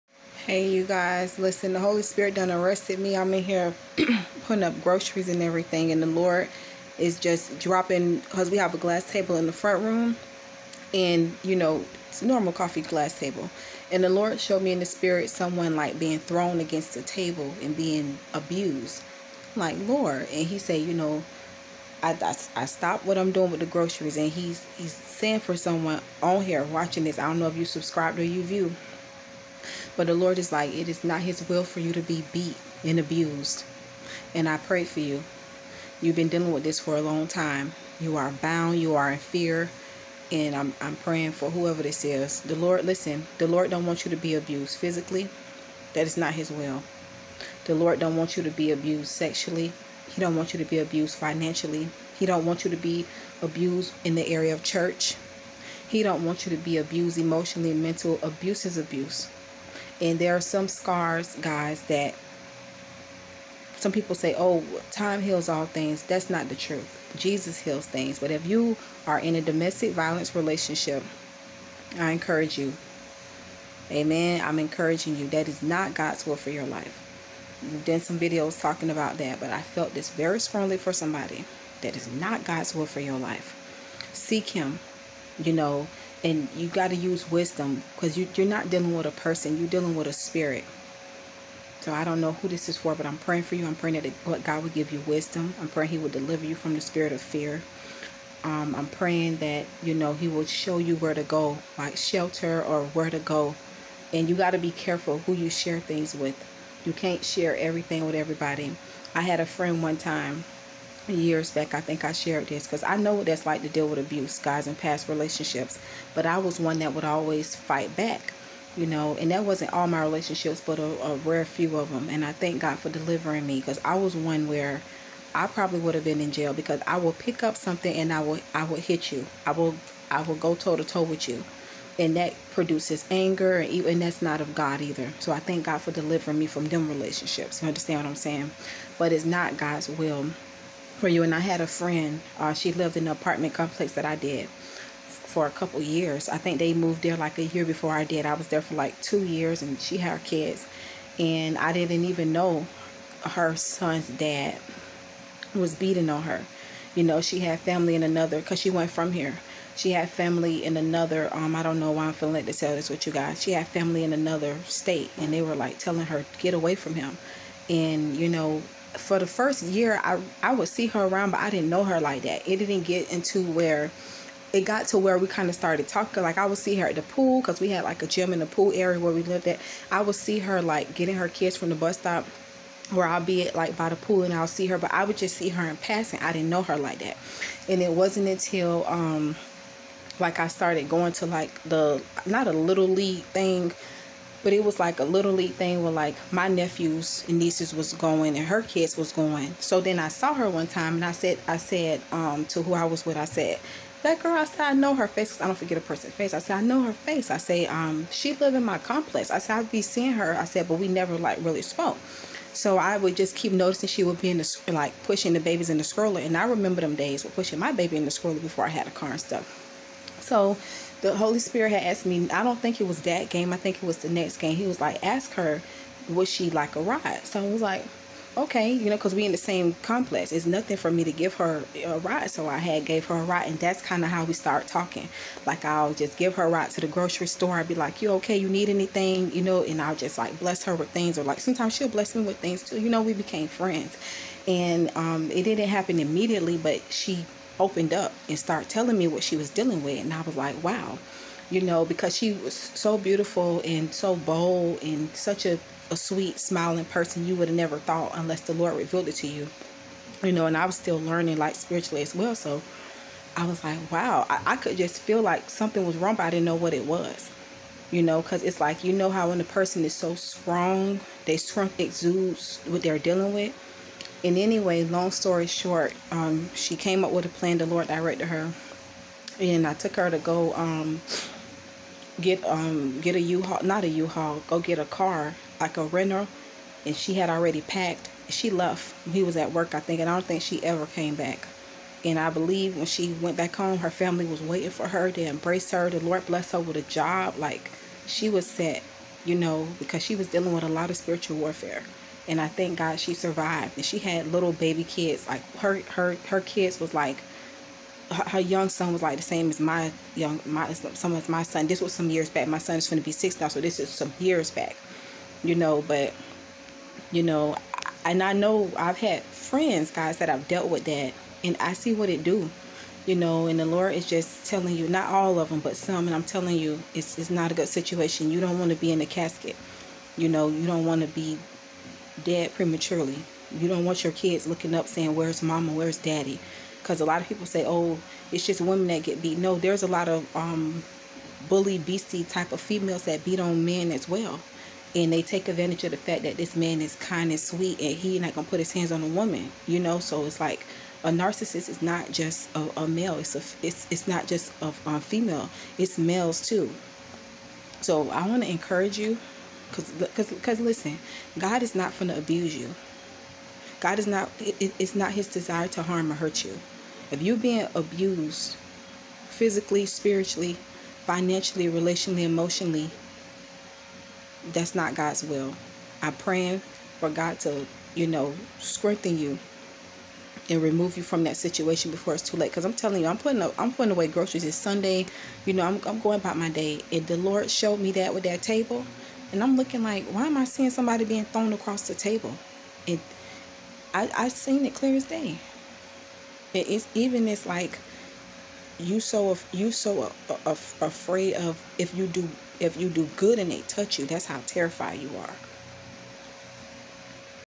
🙅BE FREE-Domestic Violence Is NOT God's Will For Your Life, Nor is Violence or Abuse of Any Kind!
I PLEAD THE BLOOD OF JESUS OVER YOUR LIFE AND DESTINY FOR WHOEVER THIS IS FOR THAT WOULD RECEIVE IT! MAY EVIL AND GENERATIONAL CURSES BE BROKEN OFF YOUR LIFE IN JESUS NAME. STEP INTO YOUR NEW! ISAIAH 43:18-19. FREEDOM OVER YOUR LIFE IN JESUS NAME! Galatians 5:1, Isaiah 26:3

It's not about what ppl think it say about you, it's what GOD Says & Thinks about you 💜

I also forgot to mention that the Lord used me to Encourage my friend with devotionals and kindness and days before she left I took her to get coats for her and the kids, groceries and boxes so that she could put it away until it was time to go get the Rental Card so she prayed, we prayed together and she moved literally in faith and also using Wisdom.

Violence is not love, abuse is not love God does not have to beat you to show you that He loves you. He will send anyone to beat you to show you that either- that is the devil! (John 10:10)  PLEASE take heed and use wisdom with this.

If you are not good to/ for yourself, you can't be good to/ for anyone else.

 Alot of times when people hit you they will say they will never do it again it was just a mistake but more than likely they will continue to do it because you allow them to do it. That creates a demonic Stronghold

I had to be obedient and share this Word 🧡 I am praying for the soul that this is for God knows I prayed for you before recording and I prayed for you immediately after.

 Don't believe the Lies of the enemy that you deserve it, that you don't or can't receive better or that you're too far gone. The devil is a liar. God has bigger and brighter and better for you. I know it's not always easy and some ppl may say easier said than done but look at the future. If you can't do it for you, do it for your family and loved ones, your Children. Some ppl may have given up on you but God hasn't. Abuse or violence of any kind is NOT God's Will for your life.

Yes, some ppl may have a turnaround where God touch that person heart and they stop and repent and there's a Testimony with that but in most cases, THAT IS NOT THE CASE!

 Please use Wisdom, seek Help and seem the hand and direction of the Lord. I pray that you feel God's love, be delivered from all demonic soul ties and evil and that God divinely position and shift you to His Divine Timetable and Will for your life. In Jesus name amen 🙏

 You Are loved. 💖💖💖💖💜💜💜💜💕💕💕💙💙💙💙💙💙 You are so very special to God. You are so valuable to Him. God had more for Him but you have to really surrender and trust Him with the Life He has for you instead of the Life that you think you have.The Lord has been showing you the signs. GOD HAS MORE FOR YOU. GOD HAS A PLAN FOR YOUR LIFE. GET OUT NOW! ⚠️⚠️⚠️⚠️

Please take heed and I pray God's perfect Love on you. Meditate on these Scriptures & Please Read Them and Receive Them: Jeremiah 29:11-13, Proverbs 3:5-6, 2nd Timothy 1:7, Psalm 91, Proverbs 16:18